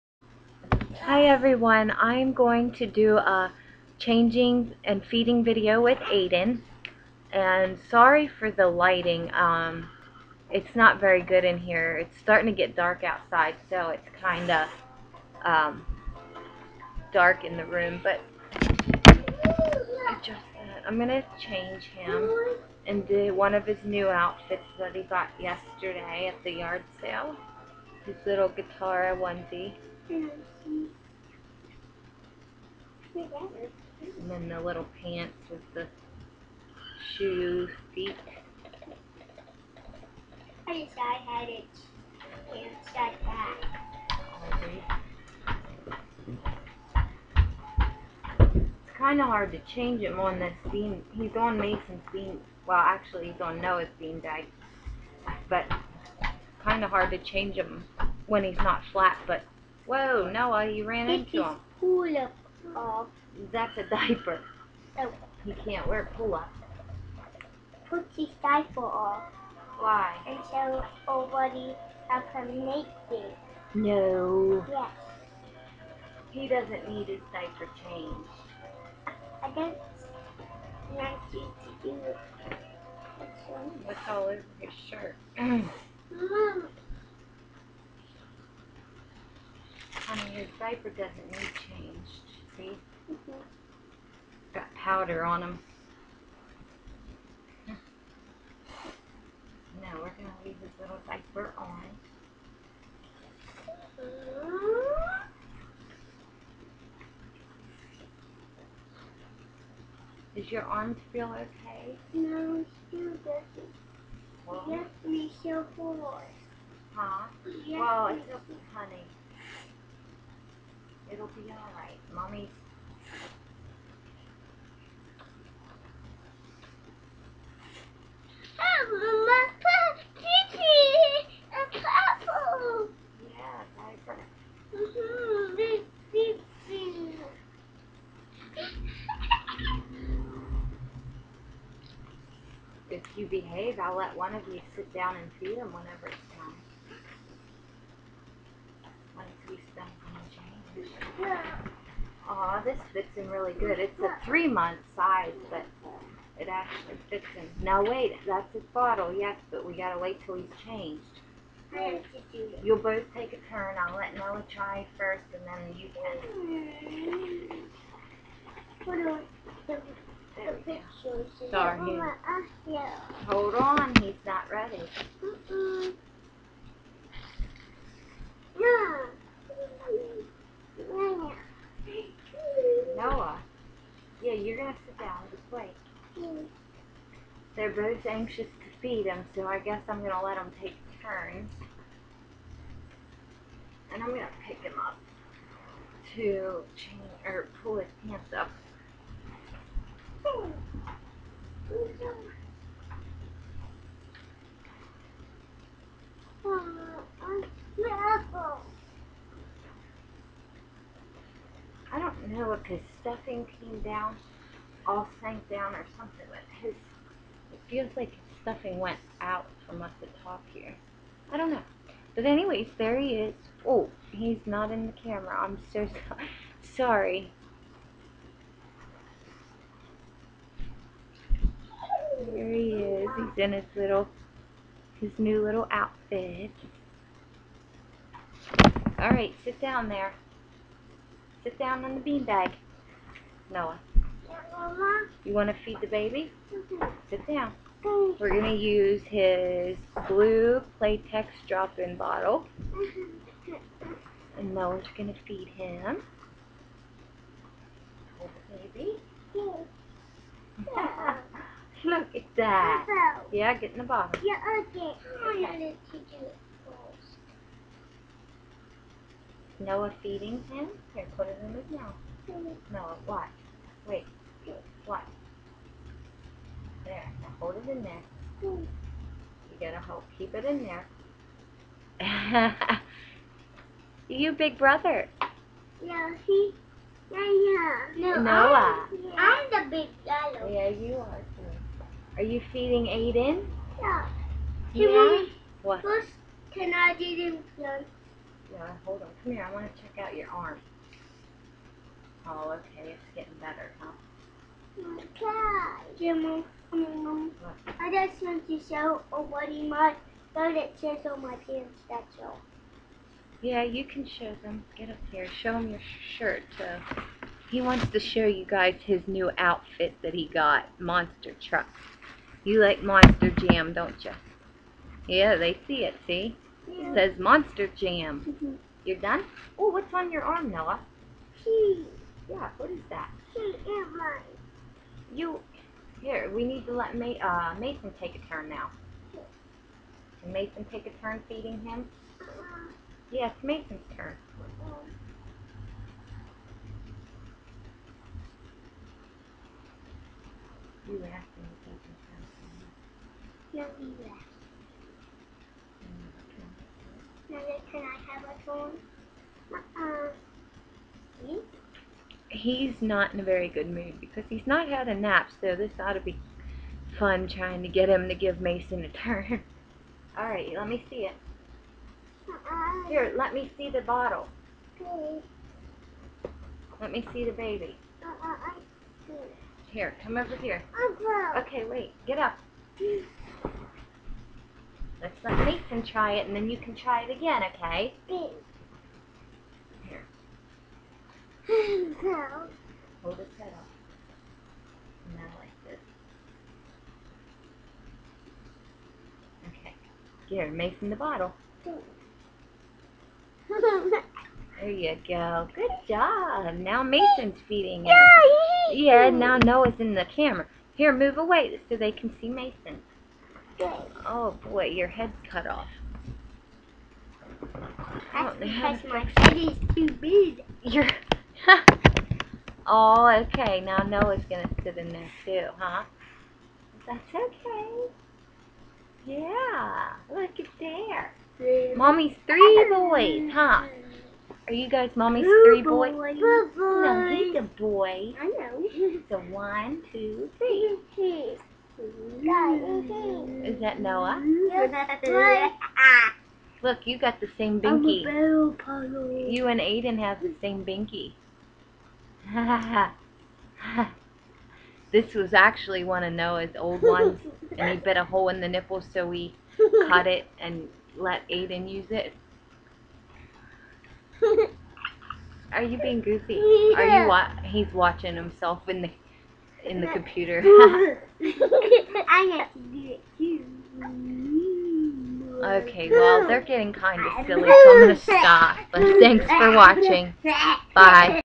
Changing and feeding reborn baby Aydin
With some help from his big brothers, Mason and Noah :D